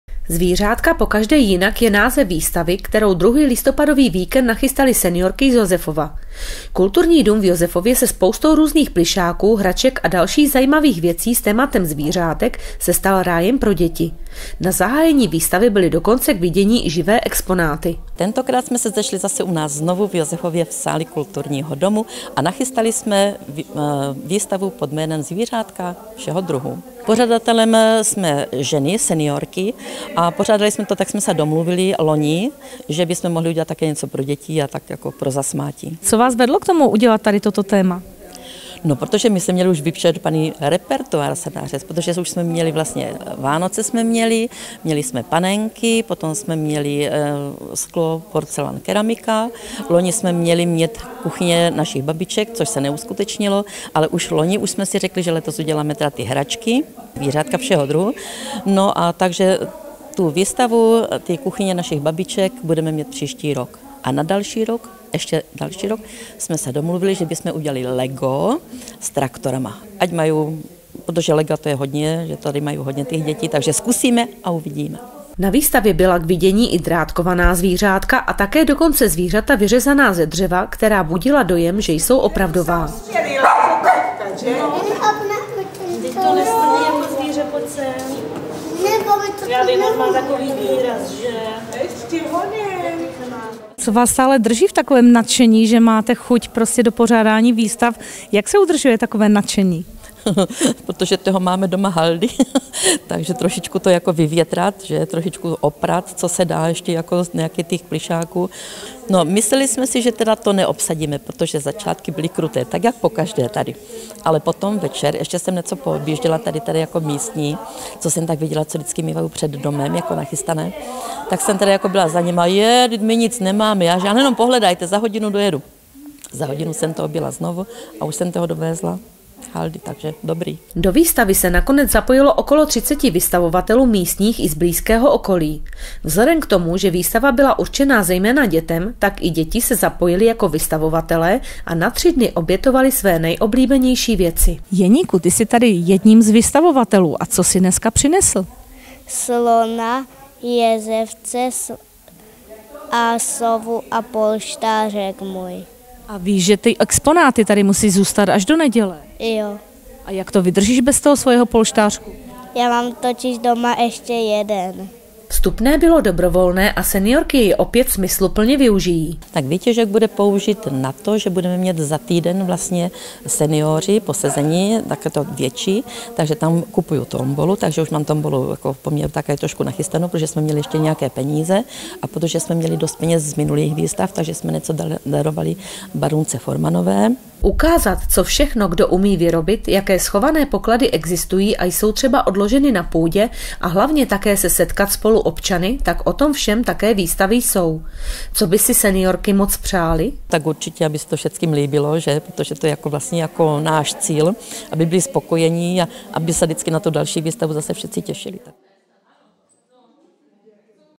Zvířátka pokaždé jinak nachystaly seniorky z Josefova
„Zvířátka pokaždé jinak“ je název výstavy, kterou druhý listopadový víkend nachystaly seniorky z Josefova. Kulturní dům v Josefově se spoustou různých plyšáků, hraček a dalších zajímavých věcí s tématem zvířátek se stal rájem pro děti. Na zahájení výstavy byly dokonce k vidění i živé exponáty.